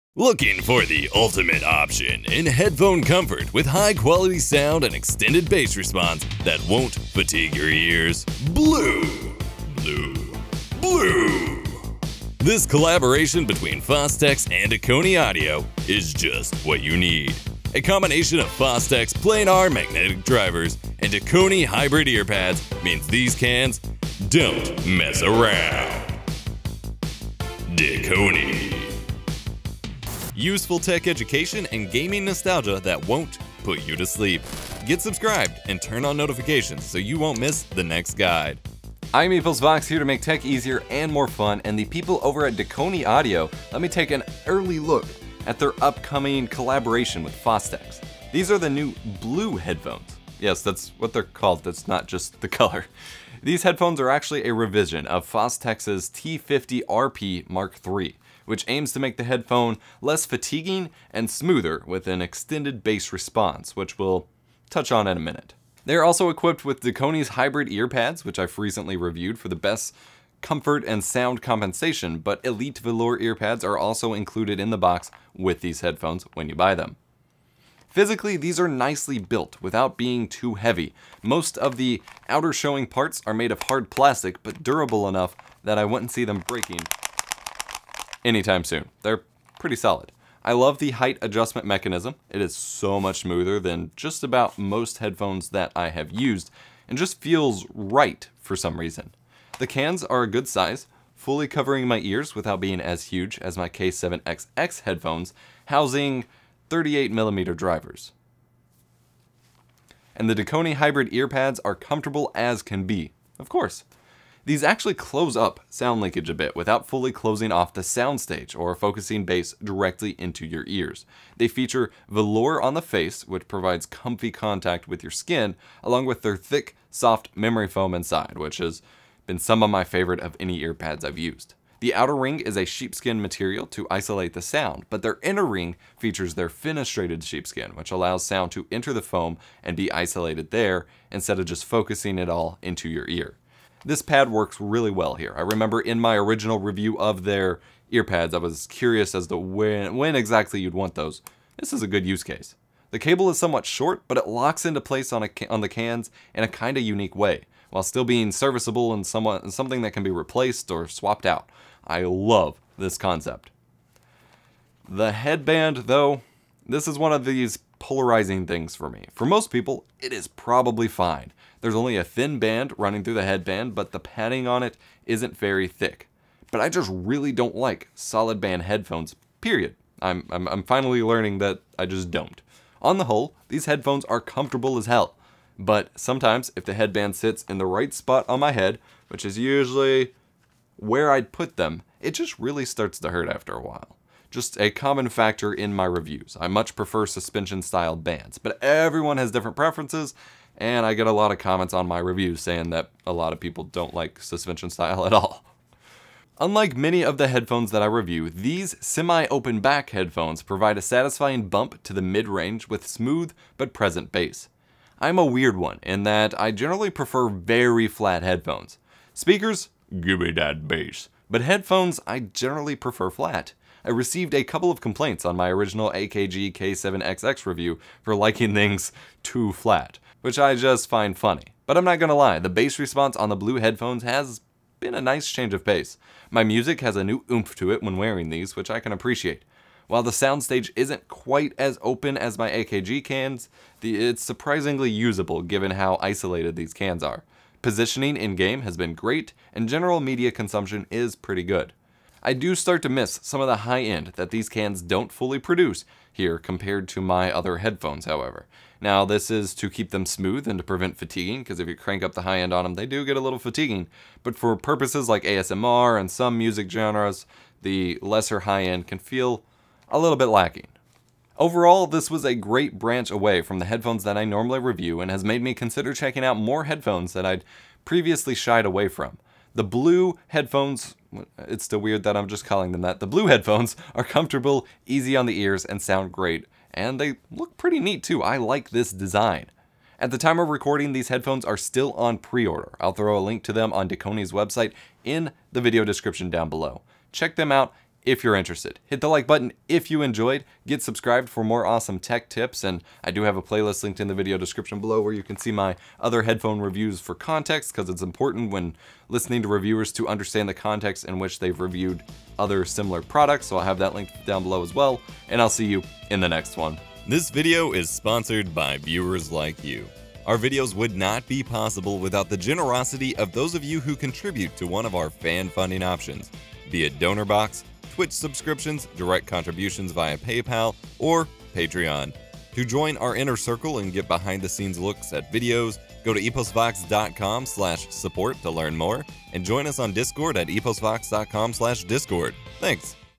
Are these your next headphones? - Dekoni Audio Blue Headphone Review | Fostex Remix
It's about time we did another headphone review... and a 90s commercial to go with it!
These are the Dekoni Audio Blue headphones - a collaboration with Fostex that provides comfort and some solid sound in a nice, stylish package. The pads are great, the sound is smooth, and I LOVE the headphone locking mechanism. I hope you enjoy my Dekoni Audio Blue review.

These are the BEST ear pads for my favorite headphones - Dekoni AKG K7xx Ear Pads Review